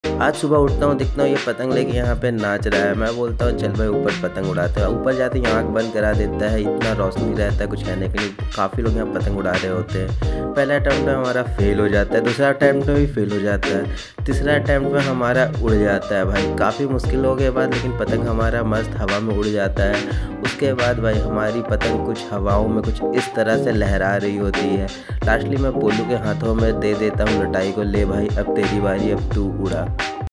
patang udaoo , fail attempt| minivlog-24
kite Lovers 🪁 vs Angry aunty 😡 funny comedy kite @